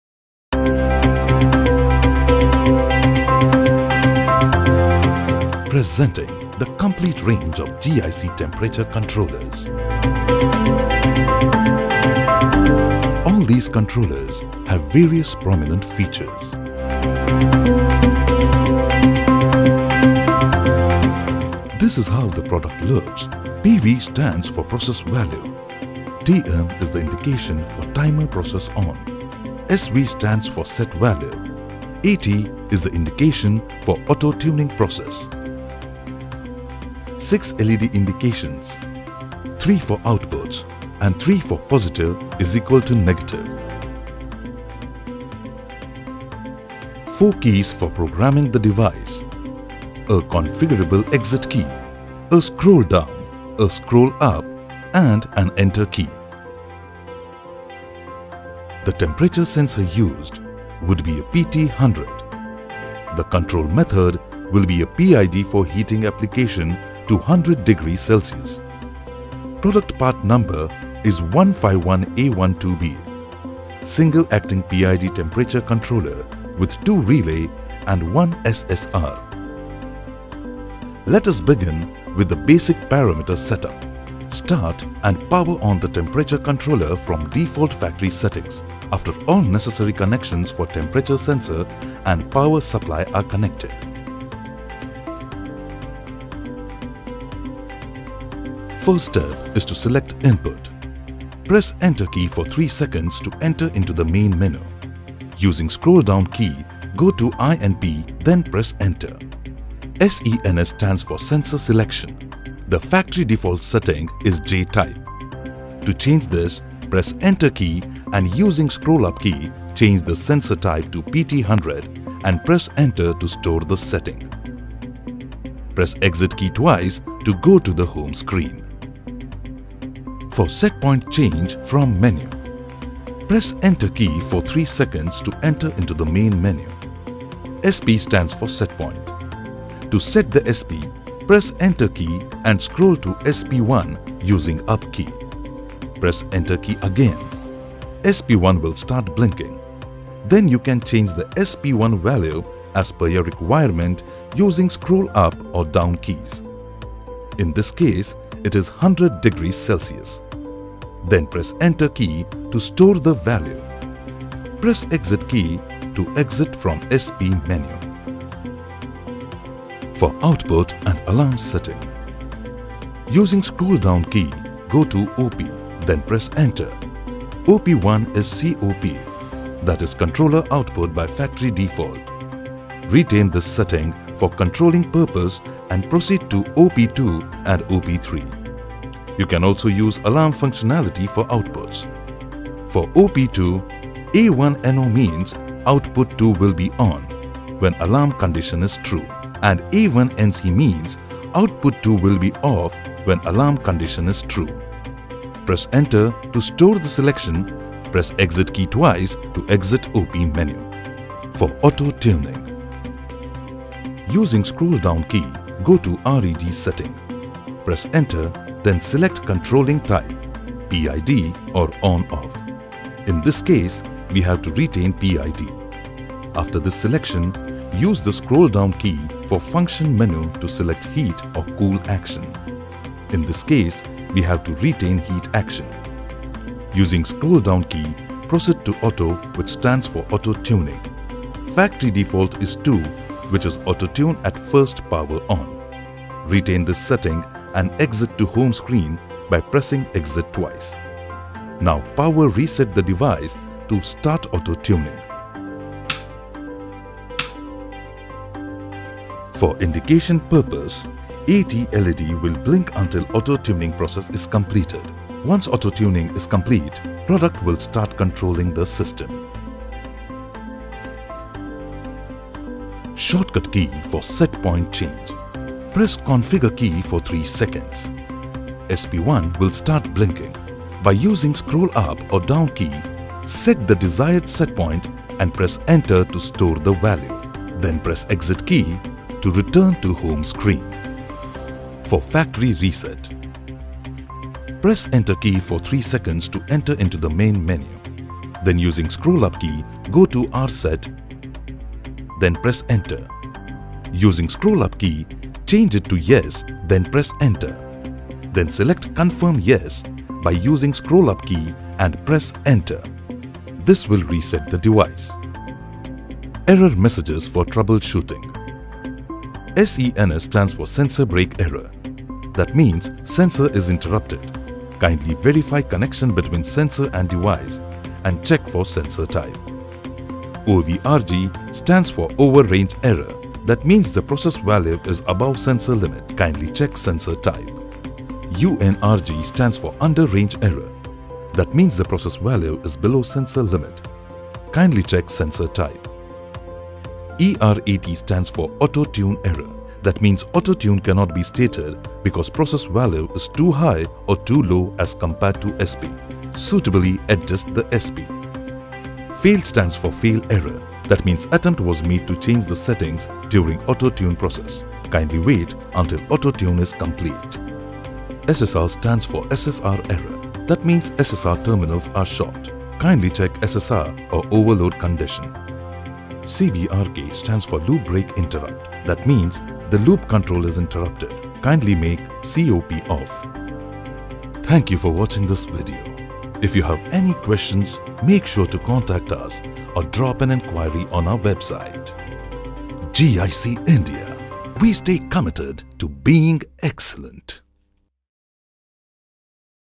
GIC Learning Hub | PID Temperature Controller PR69 | English
A comprehensive tutorial on basic programming of PR69 PID Temperature Controller in English from GIC.
Model: 151A12B Series: Advanced PID Temperature Controller Series PR69 Programming.

Timestamps:
Example - 01:04
Basic Parameter Setup - 01:23
First Step 1 (Input Sensor) - 01:38
Step 2 (Set Point) - 02:12
Step 3 (Output Setting) - 02:50
Final Step 4 (Auto Tuning) - 03:31
Shortcut Key for Set Point Change - 04:35
Factory Reset: 04:52
Error messages for Troubleshooting - 05:18

Welcome to the GIC Learning Hub series! In this episode, we bring you an in-depth guide to the PID Temperature Controller PR69. Designed to ensure precise temperature control in various applications, the PID Temperature Controller PR69 is a powerful and versatile device.

Join us in this English tutorial as we explore the fundamentals of PID temperature control and how the PID Temperature Controller PR69 simplifies the process. Learn about the key features of this digital temperature controller and its advanced PID control algorithm, enabling you to achieve optimal temperature regulation with accuracy and efficiency.

Our expert instructors will walk you through the basics of PID control, explaining the significance of proportional, integral, and derivative parameters. Discover how to program and fine-tune the PID controller parameters to suit your specific temperature control requirements.

With practical examples and step-by-step instructions, we'll demonstrate the programming and operation of the PID Temperature Controller PR69. Gain insights into its advanced functionalities, such as auto-tuning, alarms, and user-friendly interface, empowering you to maximize its potential in your applications.

Whether you're an engineer, technician, or avid learner, this tutorial is designed to equip you with the knowledge and skills to effectively utilize the PID Temperature Controller PR69. Stay ahead of temperature control challenges with GIC's trusted solutions.